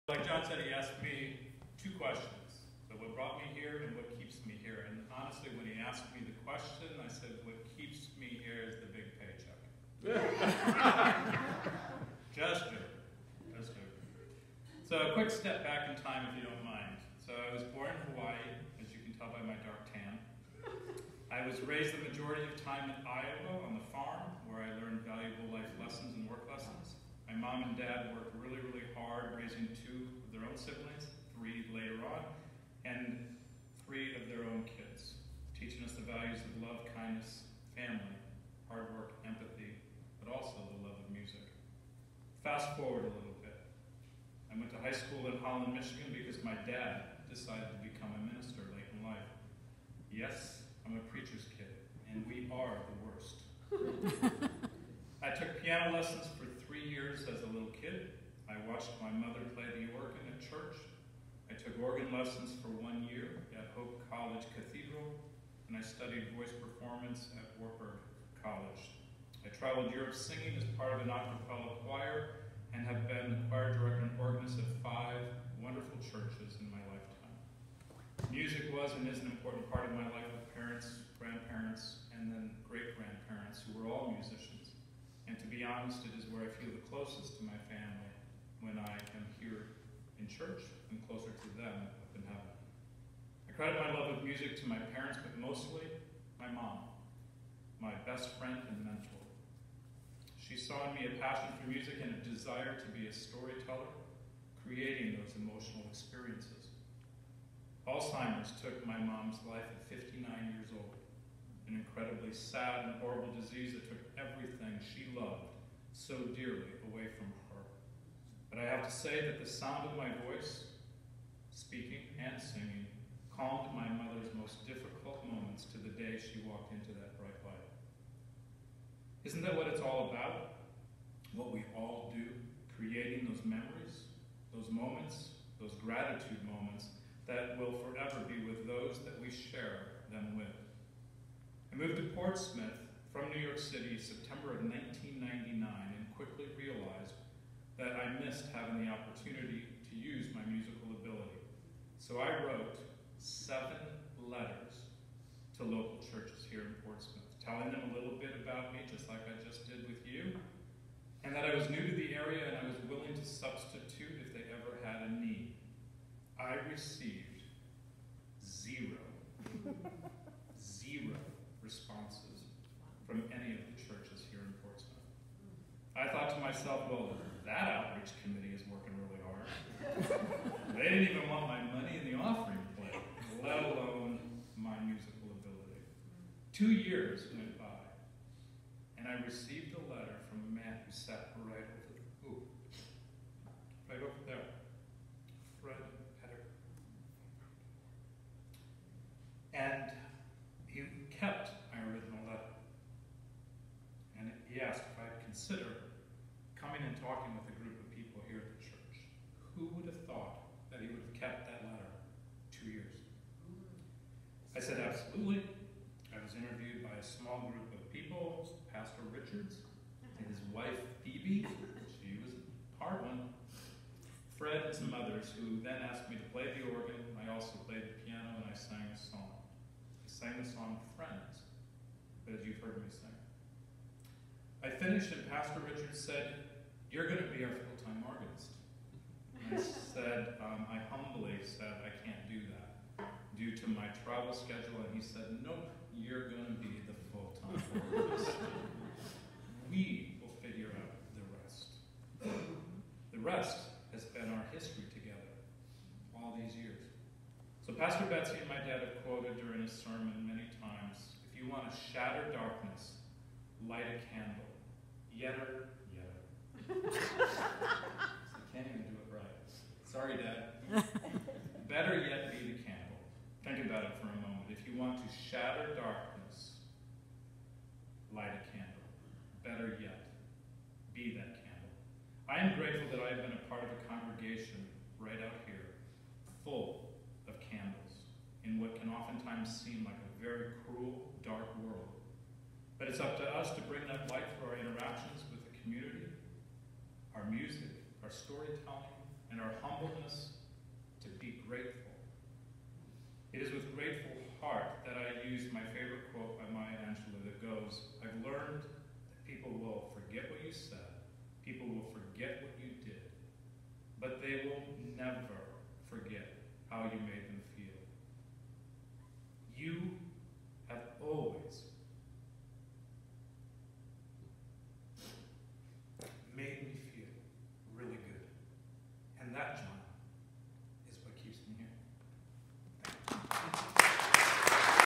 Grateful Heart Campaign
Bevan Bloemendaal speaks to the congregation answering 2 questions posed by John VanReenen for the Grateful Heart Campaign.  What brought you to First UMC and what keeps you coming back.   4/15/18